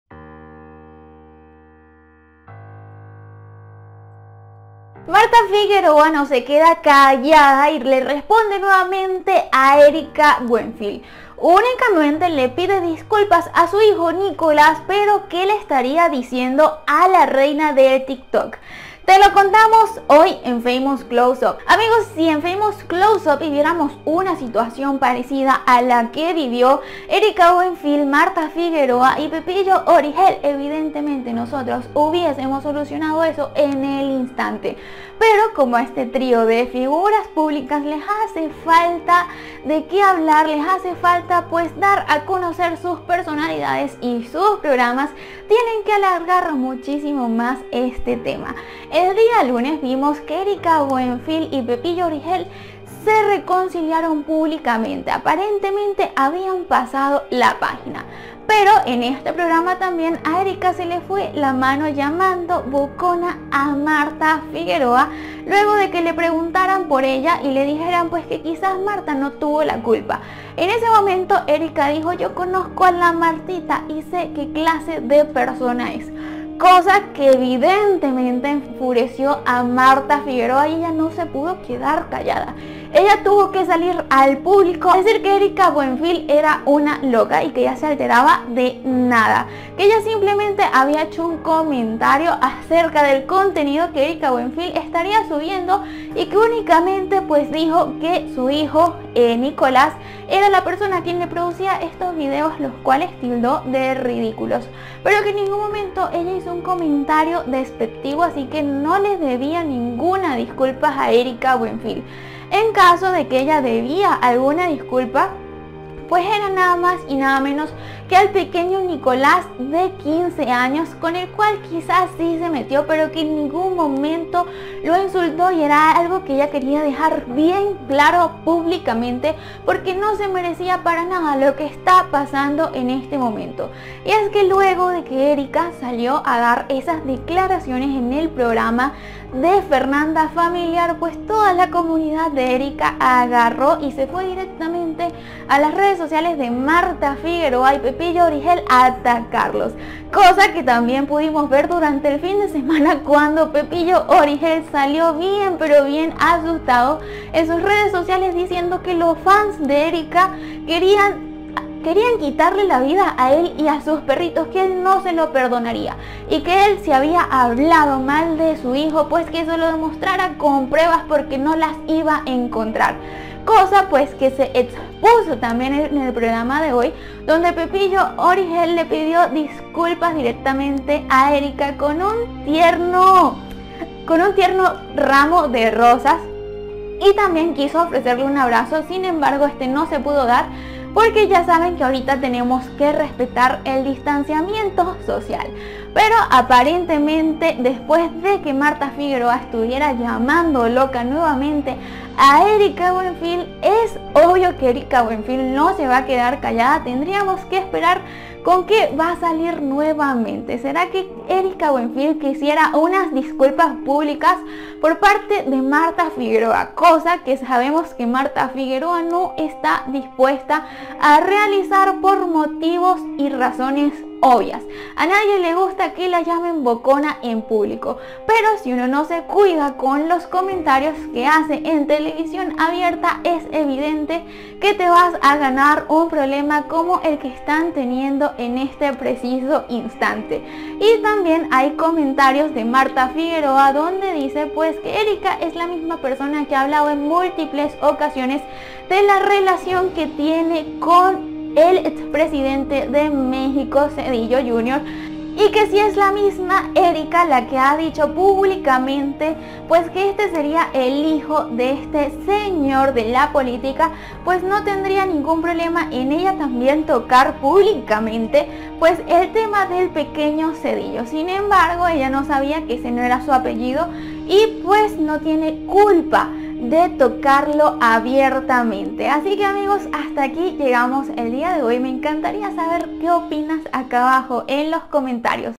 🔴😠 ¡PREPOTENTE! Martha Figueroa 𝗦𝗼𝗯𝗿𝗲 𝗹𝗮 𝗕𝘂𝗿𝗹𝗮 a Hijo de Erika Buenfil 🧐
Martha Figueroa se habría referido al joven como "El pequeño Zedillo" lo que causo molestia en su madre Erika Buenfil